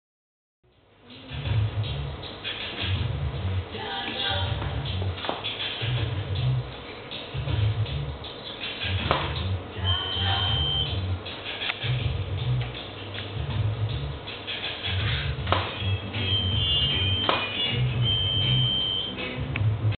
Beauty Flow Magazine backstage (Ashley Hinshaw & Marianna D)
Jean-François Verganti at work.